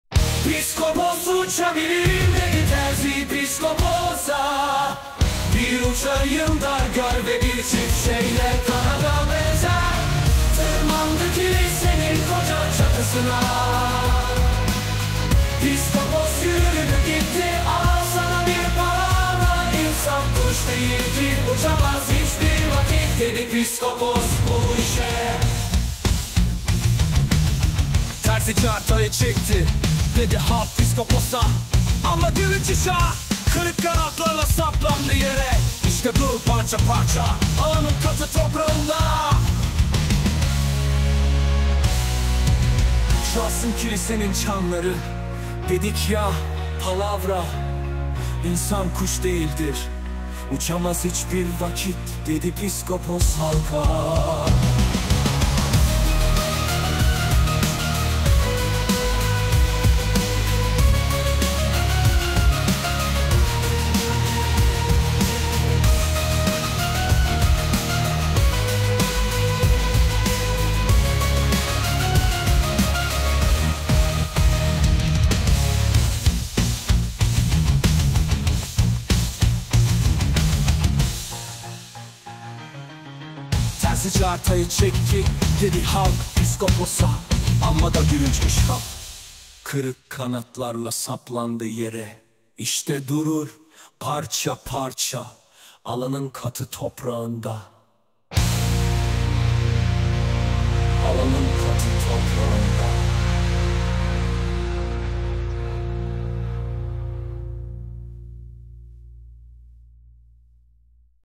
Uçan terzi / BERTOLT BRECHT / FARKLI FORMAT
Piskopos, uçabilirim,
dedi terzi piskoposa.
Bir uçayım da gör!
Ve bir çift şeyle,
kanada benzer,
tırmandı kilisenin koca çatısına.

Piskopos yürüdü gitti.
Al sana bir palavra,
insan kuş değil ki,
uçamaz hiçbir vakit,
dedi piskopos bu işe.

Terzi cartayı çekti,
dedi halk piskoposa,
amma da gülünç iş ha,
kırık kanatlarla saplandı yere,
işte durur parça parça
alanın katı toprağında.

Çalsın kilisenin çanları!
dedik ya, palavra,
insan kuş değildir,
uçamaz hiçbir vakit,
dedi piskopos halka.